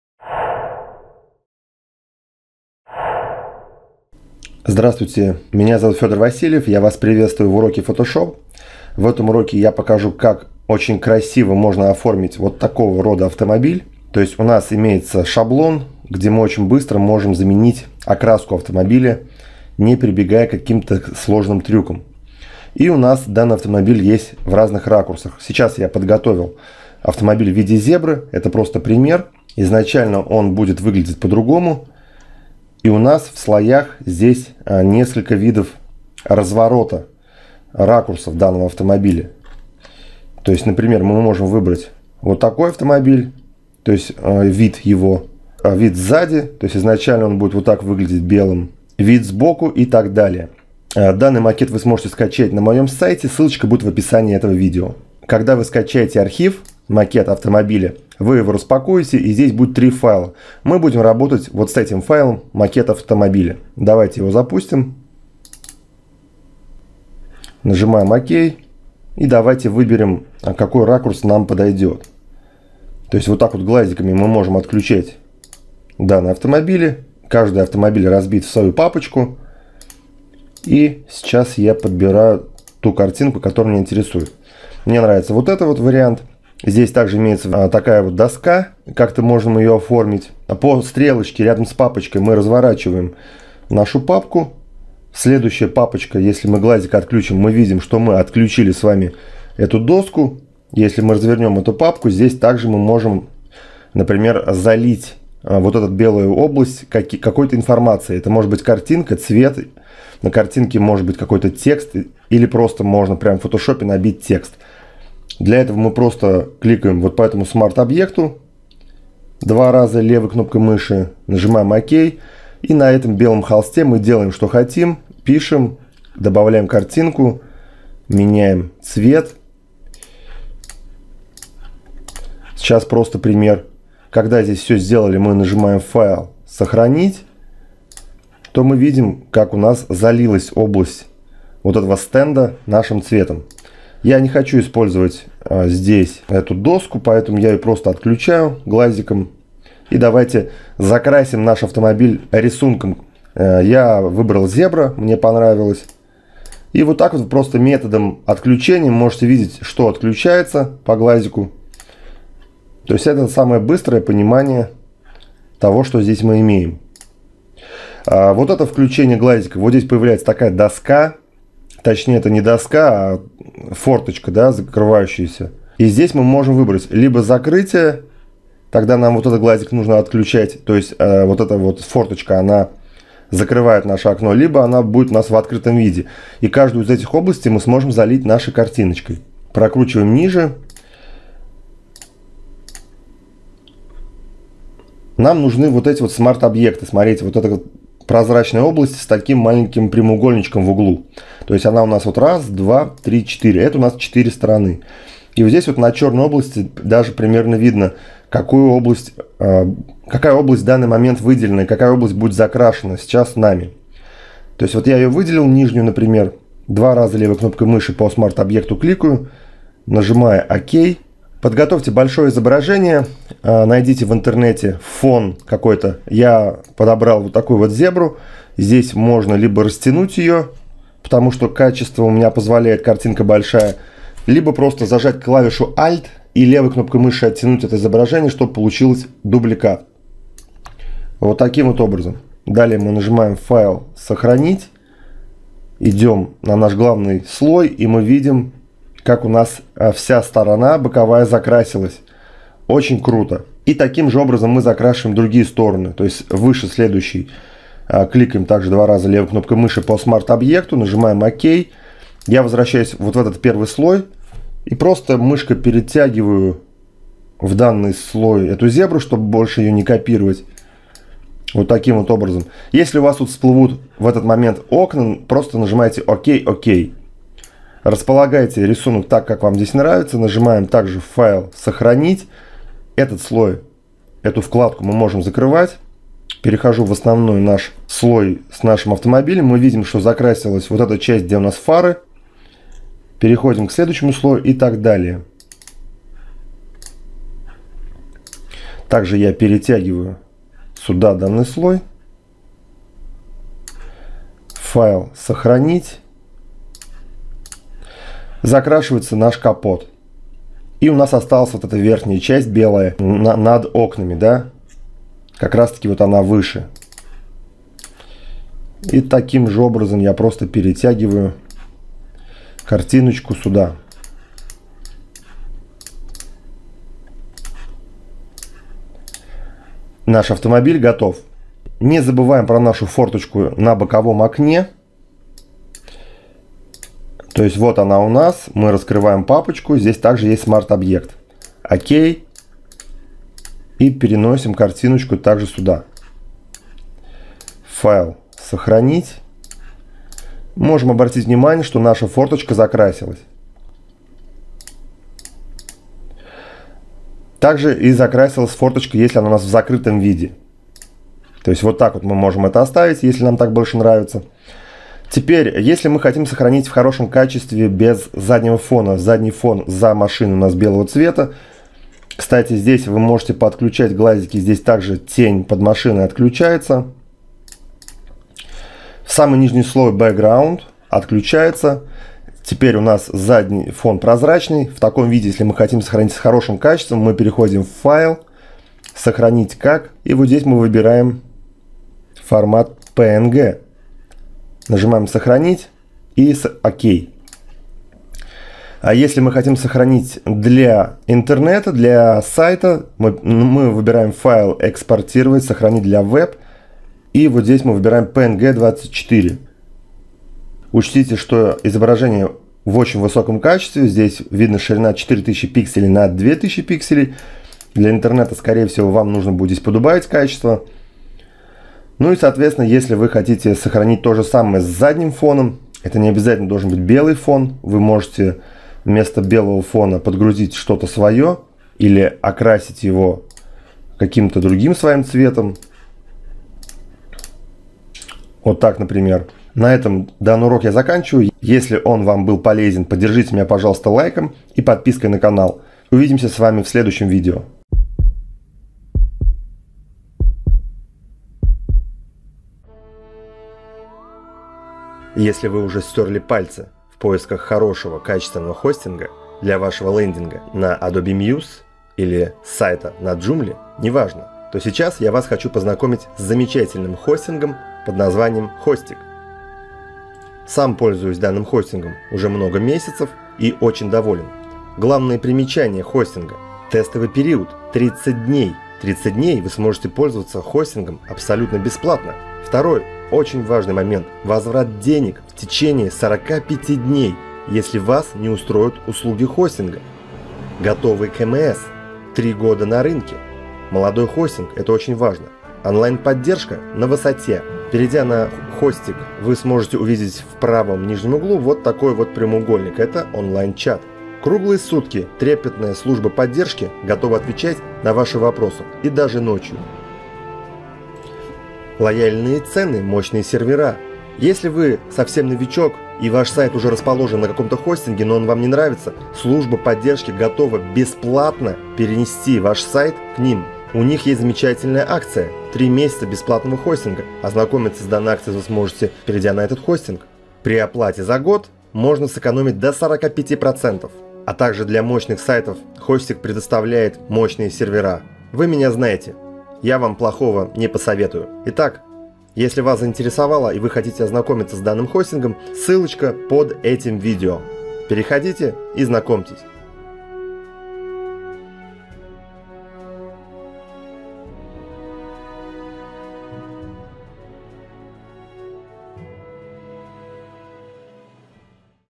4.Макет автомобиля в фотошопе | Mock Ups
В этом уроке я вам покажу как можно в фотошоп 2017, очень быстро, окрасить автомобиль в любой окрас использую готовый макет автомобиля.